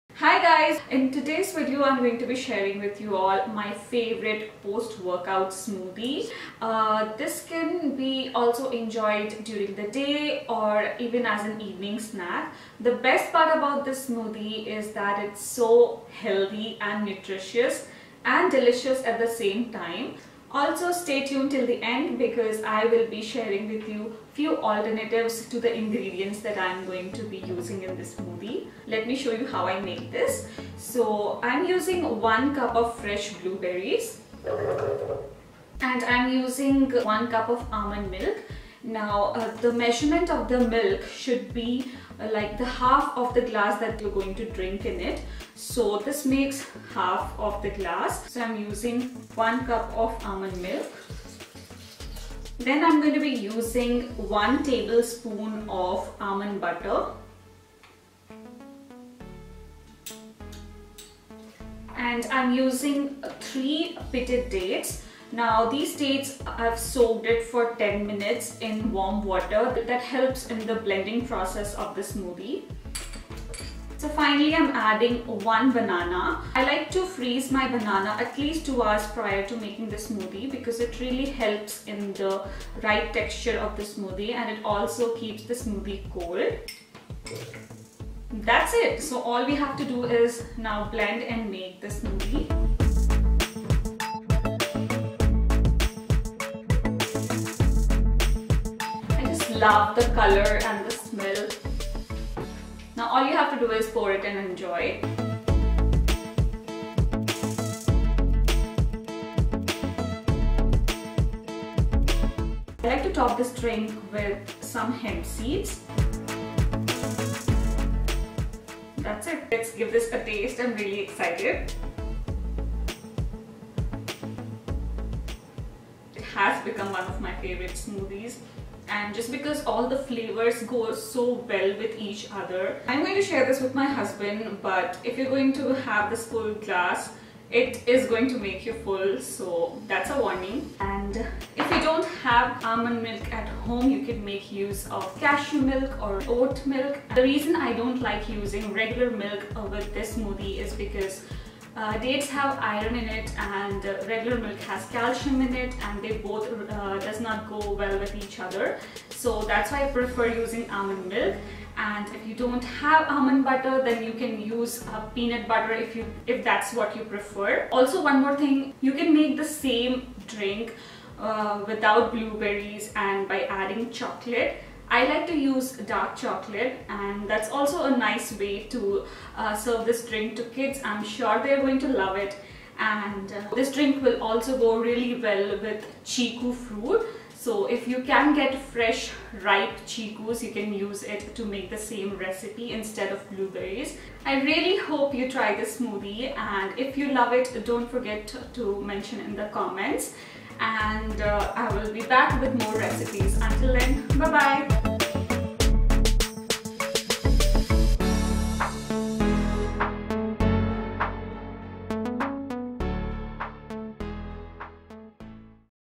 Vegan Blueberry Almond Smoothie/ Post workout smoothie
Hey guys, I'm sharing my favorite post workout smoothie. It's vegan but very delicious. I have also suggested few alternatives to enjoy this drink towards the end of this video.
The ingredients in this smoothie are packed with energy and nutrition.

Notes:
1. You can add additional protein powder if you like but in my opinion this drink is already heavy and does not need any.
2. The color of the smoothie changes soon if you don't drink immediately but the taste remains the same (soo yummyyy!!)
 3. Don't forget to try the same smoothie with the alternatives suggested at the end. They are also my favorite combinations. :)

Love
Sandy